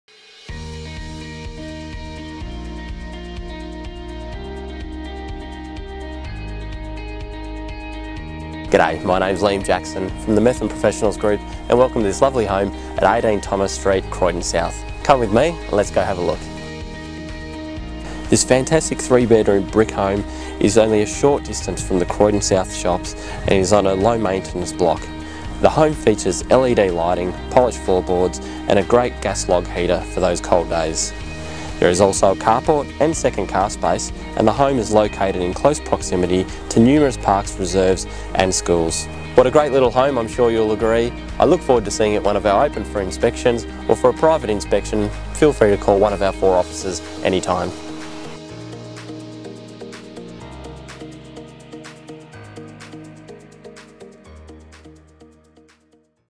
18 Thomas Street, Croydon South
This neat three bedroom home offers a huge lounge with gas log heater and the timeless appeal of polished floorboards. The low maintenance block includes a vegie patch, two sheds and a covered front deck. Best of all is its enviable central location and close proximity to schools, kinders, shops and parks!